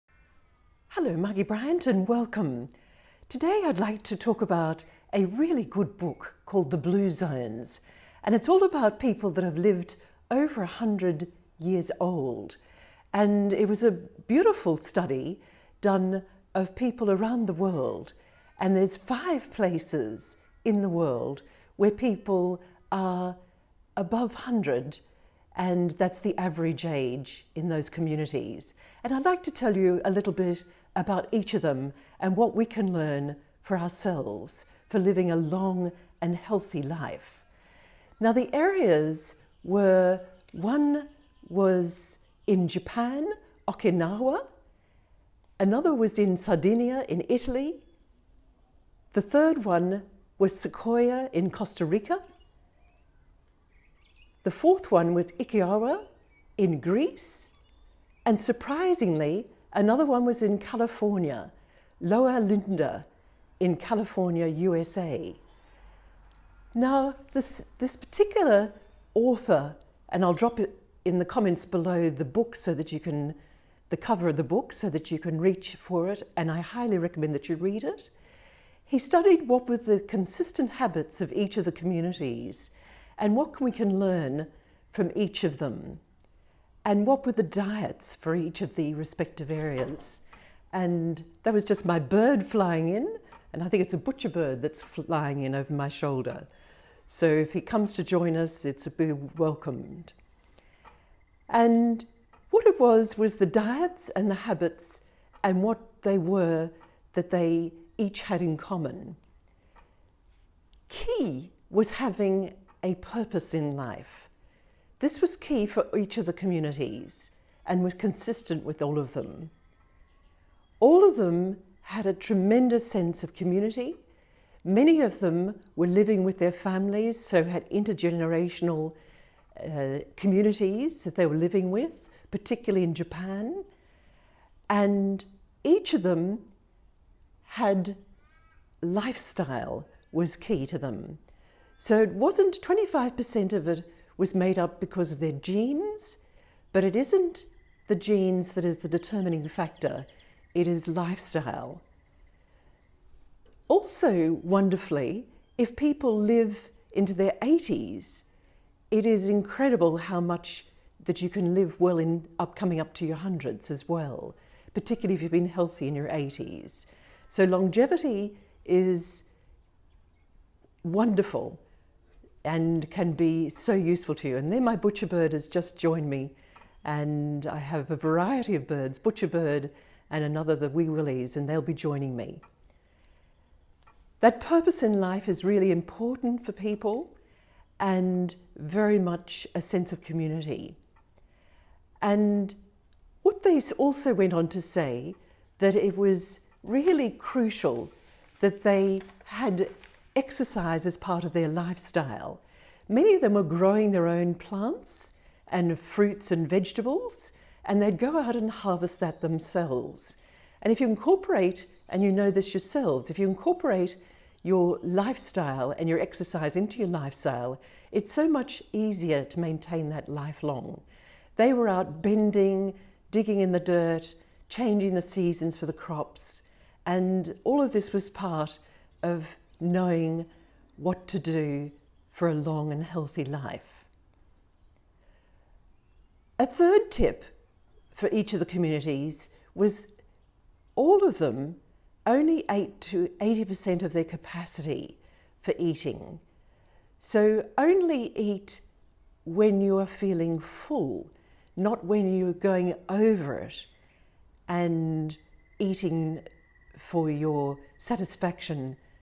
Eating and Living Like The World's Healthiest People
Eating and Living Like the World's Healthiest People. Find out where people live in great health to over 100, what they eat. What are their habits in The Blue Zones and what are the lessons for us for living well longer?